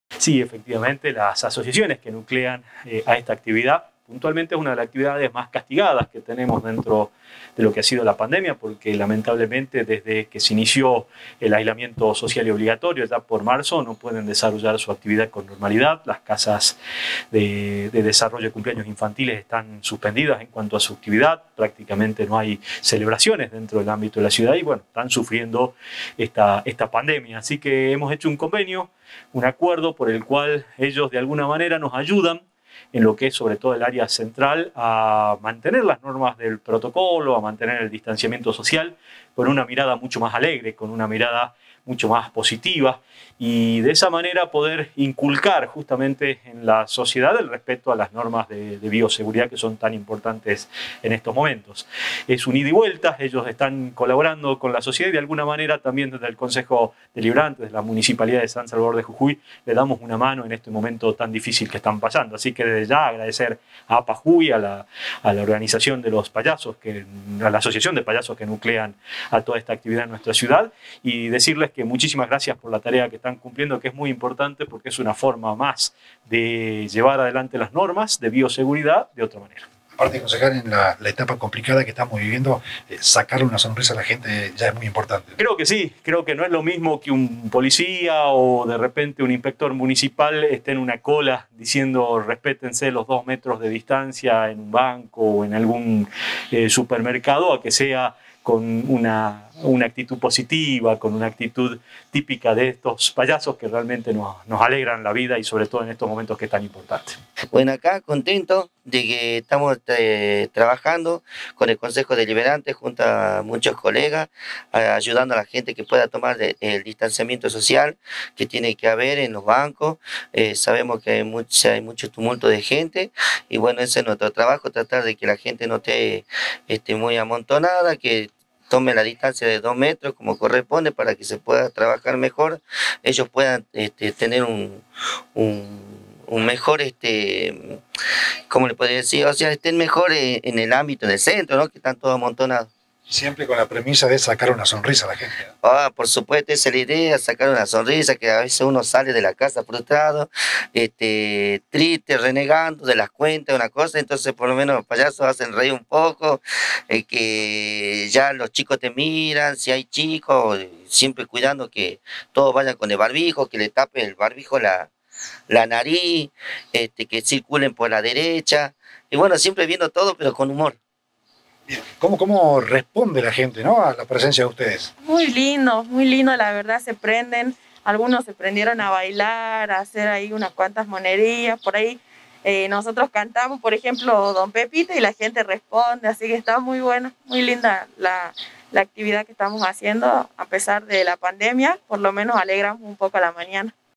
CONCEJALES DESTACARON EL TRABAJO QUE VIENEN REALIZANDO PAYASOS Y PAYASAS EN EL MARCO DE LA PANDEMIA
Ediles capitalinos recibieron esta mañana a diferentes payasos y payasas con el objetivo de agradecer y resaltar el trabajo que vienen realizando en el marco de la pandemia, ayudando en zonas céntricas para que la gente respete las medidas de bioseguridad.

El presidente de la institución, Lisandro Aguiar y la concejal Patricia Moya fueron los encargados de recibirlos. Se hicieron presentes: el Mago Payaso Ilusionista “Nino” Héctor Soria, el Payaso Mago “Pastelito” Julio Martínez, el Payaso “Fofito” Rafael Tolaba, la Maga Payasa “Mimi” Patricia Donaire, el Payaso “Yogursito” Miguel Ángel Ruiz y la Payasa “Cariñito” Carla Cruz.

Al respecto, el presidente de la institución, Lisandro Aguiar, afirmó que “es una de las actividades más castigadas por la pandemia, porque desde que se inició el aislamiento social y obligatorio por el mes de marzo no pueden desarrollar su actividad con normalidad”.

Asimismo, agregó: “las casas de cumpleaños infantiles están  suspendidas en cuanto a su actividad, prácticamente no hay celebraciones dentro del ámbito de la ciudad, así que están sufriendo esta pandemia. Hicimos un acuerdo, un convenio, por el cual ellos de alguna manera nos ayudan, sobre todo en el área central, a mantener las normas del protocolo, el distanciamiento social, con una mira más alegre, positiva”.